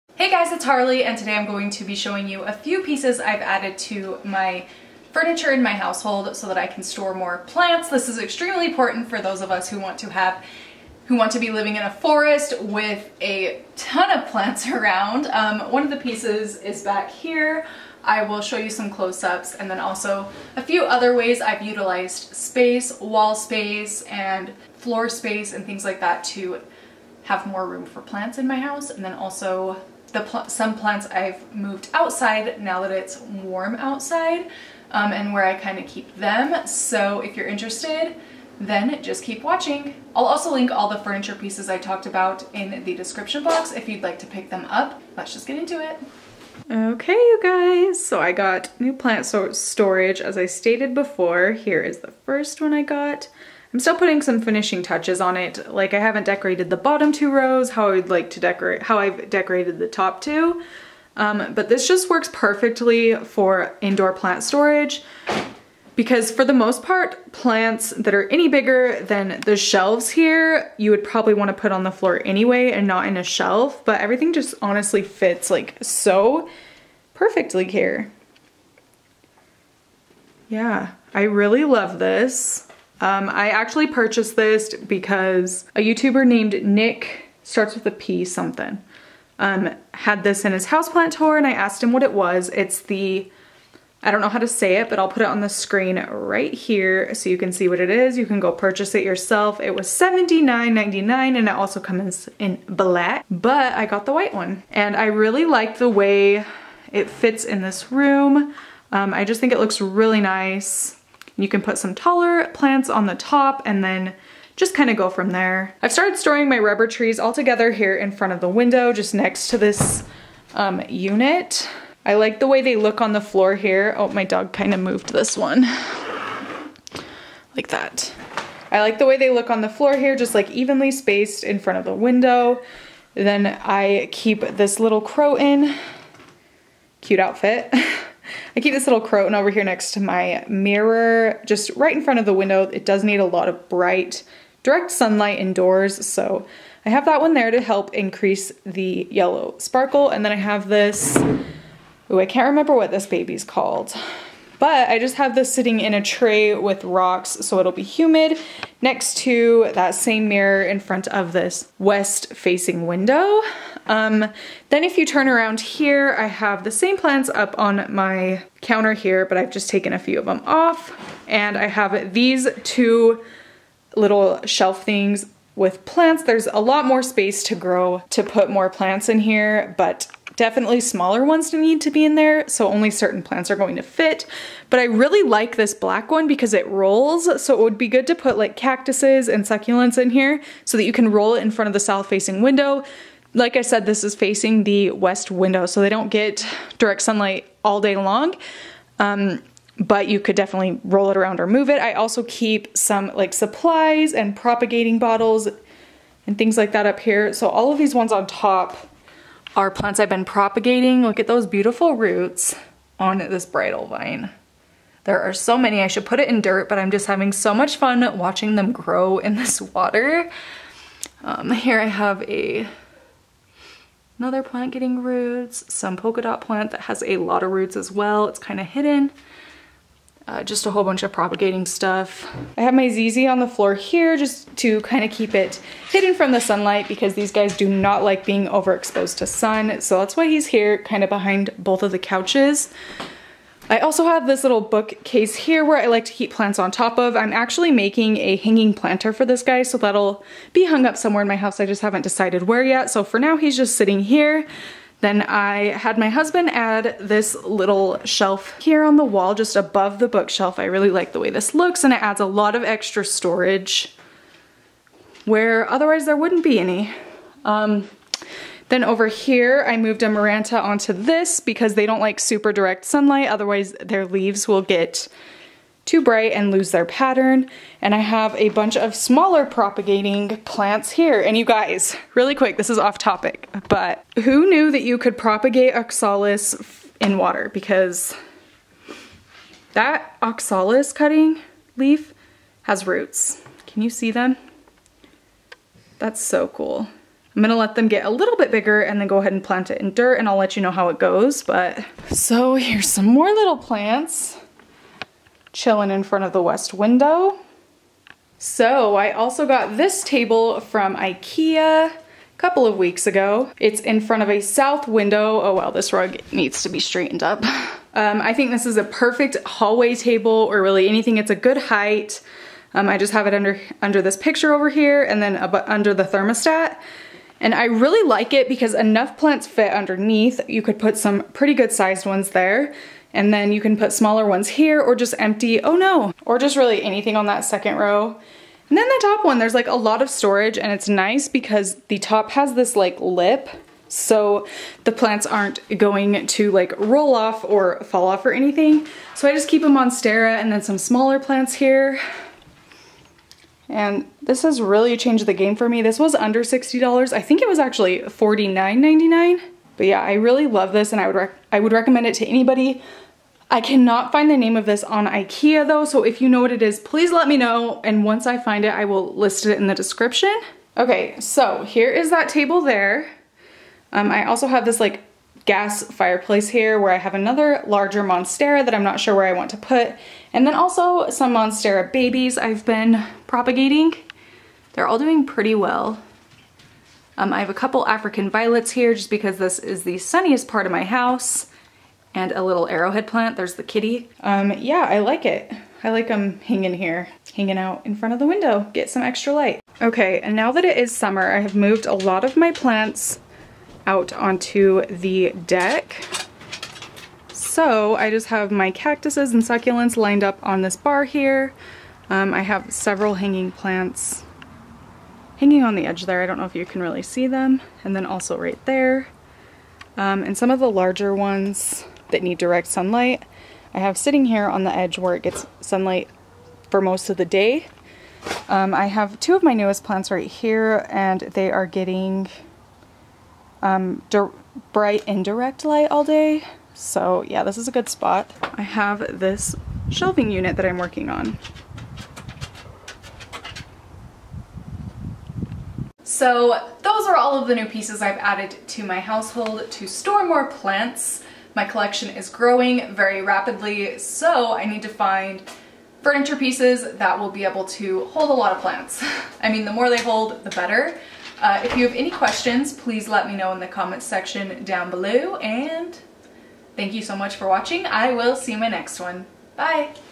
Houseplant Storage Furniture! My Favorite Plant Furniture
Today I thought I would show you what furniture I have to store all of my houseplants. I have a rapidly growing houseplant collection so storage of all the plants is vital! Them ore surface space in my home, the better in my opinion!

Products Mentioned:

White Vittsjo Shelves

Satsumas Plant Table

3 Tier Corner Shelf (not exactly the same, but I just ordered this one :P)

Rolling Storage Cart

Round Silver Side Table

Outdoor Storage (similar to mine)